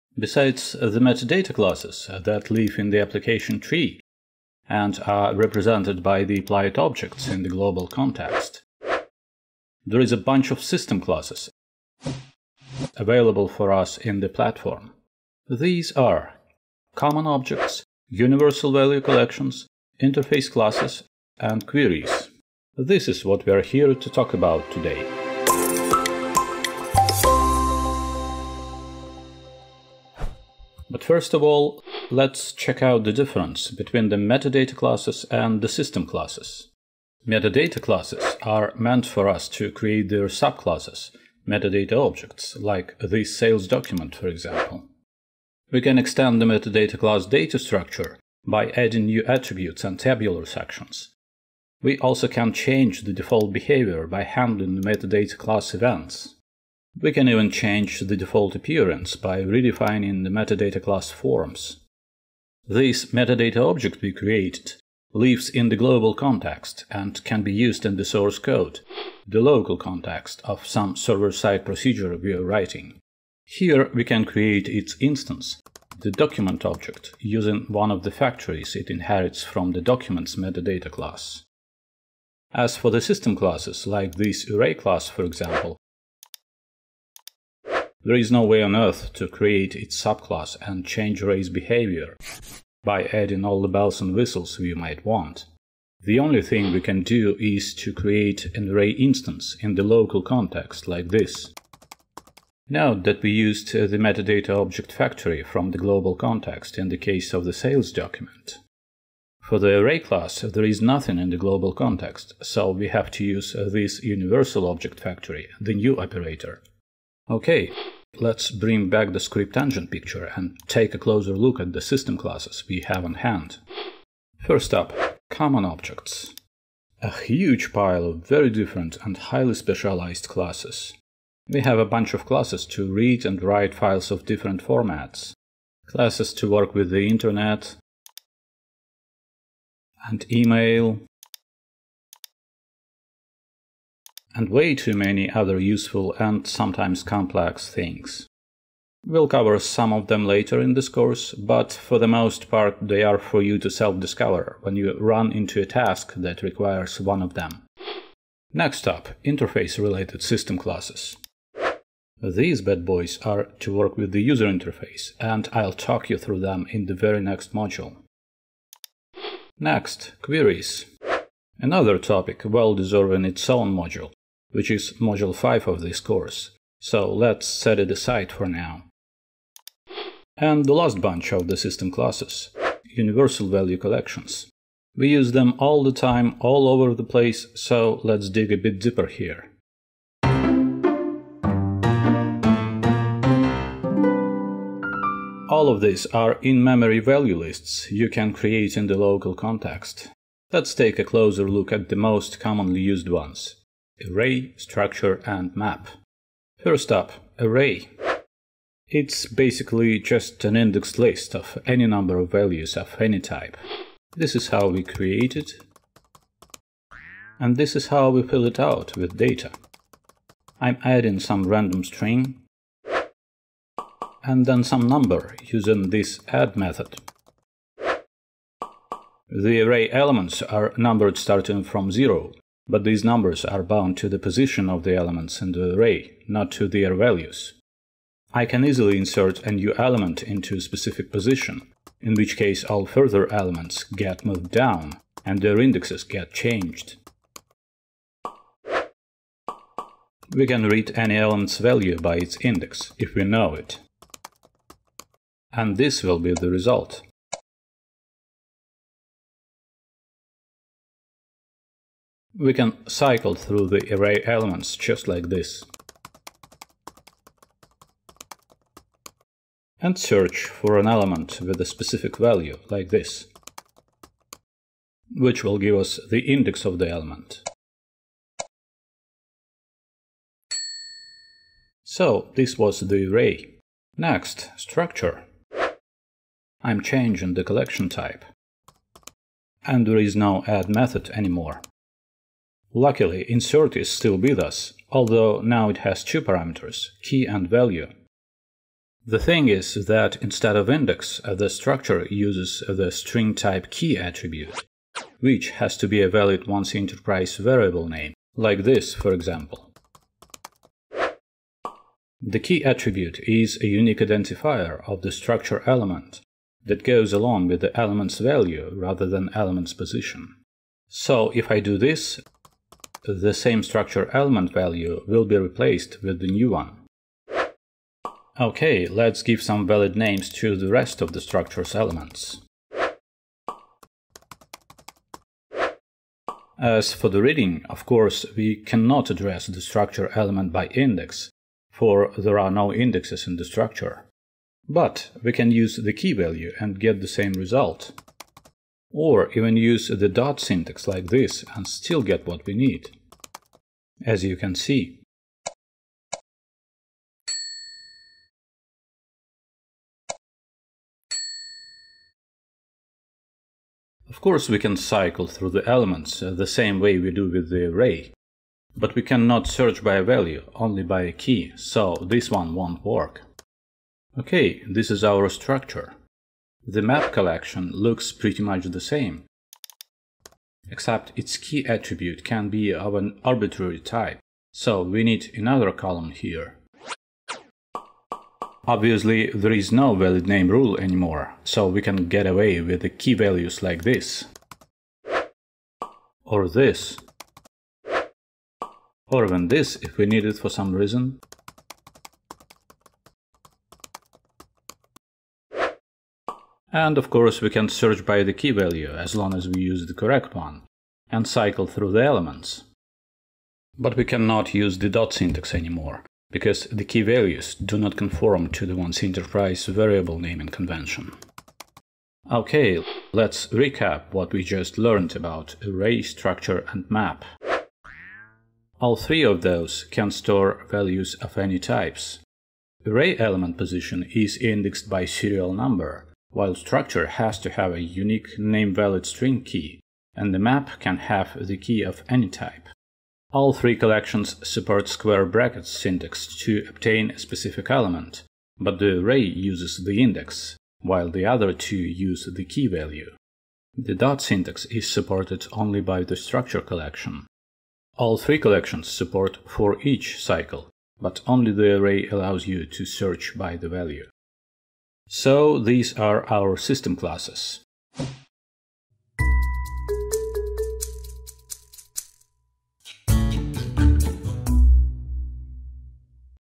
Module 3. Episode 5. System classes
1C Junior Developer training course
Module 3. 1C script language basics

Besides the Metadata Classes that live in the application tree and are represented by the Applied Objects in the Global Context, there is a bunch of system classes available for us in the Platform.

These are common objects, universal value collections, interface classes, and queries. This is what we are here to talk about today.

But first of all, let’s check out the difference between the Metadata Classes and the System Classes. Metadata Classes are meant for us to create their subclasses - Metadata Objects - like this Sales document, for example.

We can extend the Metadata Class data structure by adding new attributes and tabular sections. We also can change the default behavior by handling the Metadata Class events. We can even change the default appearance by redefining the Metadata Class forms.

This Metadata Object we created lives in the Global Context and can be used in the source code - the local context of some server-side procedure we’re writing. Here we can create its instance - the document object - using one of the factories it inherits from the Documents Metadata Class.

As for the System Classes - like this Array class, for example - there is no way on Earth to create its subclass and change Array’s behavior by adding all the bells and whistles we might want. The only thing we can do is to create an Array instance in the local context like this.

Note that we used the Metadata Object factory from the Global Context in the case of the Sales document. For the Array class, there is nothing in the Global Context, so we have to use this universal object factory - the New operator.

OK, let’s bring back the Script Engine picture and take a closer look at the System Classes we have on hand.

First up: Common objects. A huge pile of very different and highly specialized classes. We have a bunch of classes to read and write files of different formats, classes to work with the Internet and email, and way too many other useful and sometimes complex things.

We’ll cover some of them later in this course, but for the most part, they are for you to self-discover when you run into a task that requires one of them.

Next up: Interface-related system classes. These bad boys are to work with the user interface, and I’ll talk you through them in the very next module.

Next: Queries. Another topic well deserving its own module, which is Module 5 of this course.

So, let’s set it aside for now. And the last bunch of the System Classes: Universal value collections. We use them all the time all over the place, so let’s dig a bit deeper here.

All of these are in-memory value lists you can create in the local context. Let’s take a closer look at the most commonly used ones: Array, Structure, and Map.

First up: Array.

It’s basically just an indexed list of any number of values of any type. This is how we create it, and this is how we fill it out with data. I’m adding some random string and then some number using this Add method.

The Array elements are numbered starting from zero, but these numbers are bound to the positions of the elements in the array - not to their values. I can easily insert a new element into a specific position, in which case all further elements get moved down, and their indexes get changed.

We can read any element’s value by its index if we know it. And this will be the result.

We can cycle through the array elements just like this. And search for an element with a specific value like this, which will give us the index of the element.

So, this was the Array. Next: Structure.

I’m changing the collection type, and there is no Add method anymore. Luckily, Insert is still with us, although now it has two parameters: Key and Value. The thing is that instead of Index, the Structure uses the string-type Key attribute, which has to be a valid 1C:Enterprise variable name. Like this, for example.

The Key attribute is a unique identifier of the Structure element that goes along with the element’s value (rather than element’s position). So, if I do this, the same Structure element value will be replaced with the new one.

OK. Let’s give some valid names to the rest of the Structure’s elements. As for the reading, of course, we cannot address the Structure element by Index for there are no indexes in the Structure. But we can use the Key value and get the same result. Or even use the dot syntax like this and still get what we need, as you can see.

Of course, we can cycle through the elements the same way we do with the Array, but we cannot search by a value - only by a Key. So this one won’t work.